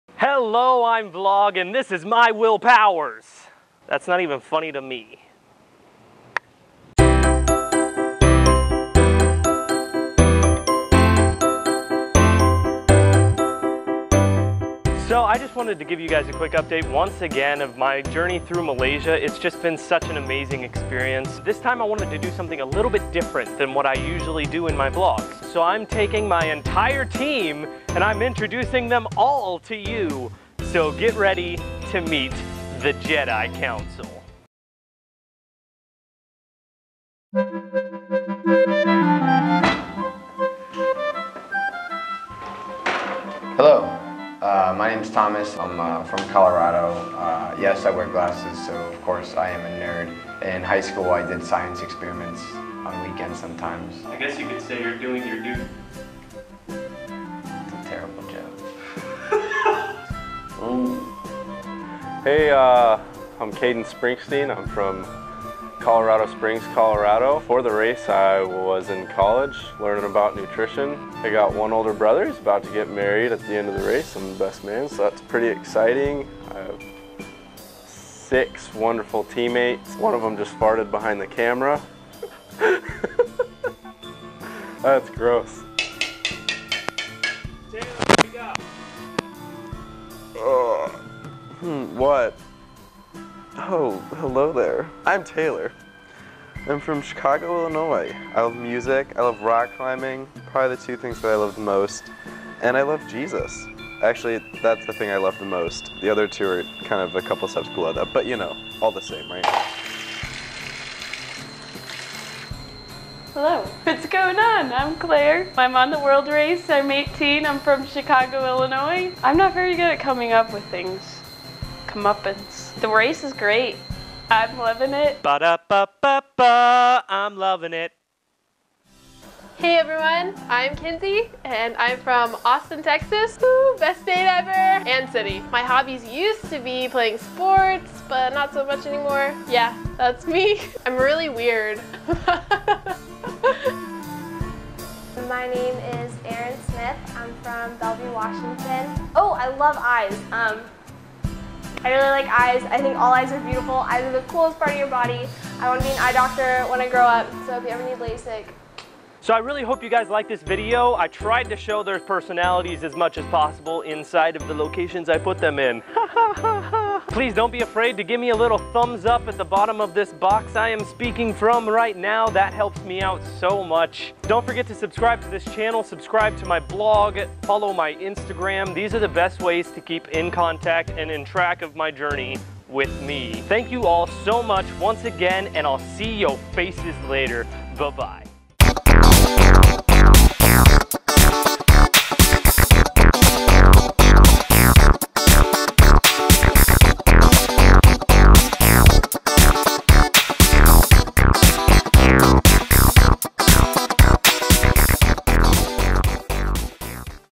Meet My Team
These are the hooligans I have spent the last month with, You can connect to all there blogs through mine to stalk them some more!